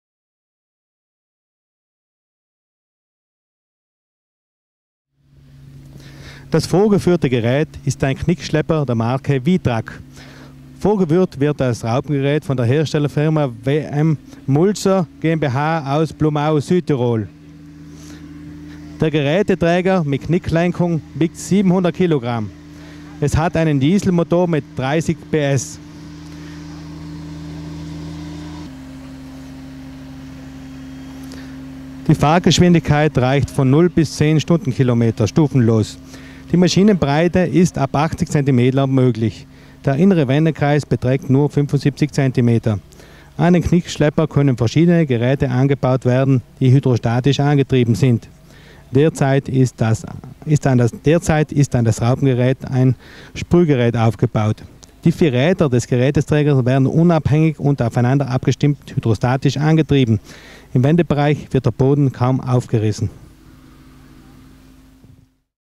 Geräteträger VITRAC: Tag der Technik, Laimburg
Auszug Film Maschinen im Vergleich, Tag der Technik Weinbau

▬ Product ▬▬▬▬▬▬▬▬▬▬▬▬▬▬▬▬▬

Slope-tool carrier

▬ Manufacturer ▬▬▬▬▬▬▬▬▬▬▬▬▬▬▬

WM technics, Blumau, Italy

▬ Special tanks ▬▬▬▬▬▬▬▬▬▬▬▬▬▬▬

Location: Laimburg

▬ Social Media ▬▬▬▬▬▬▬▬▬▬▬▬▬▬▬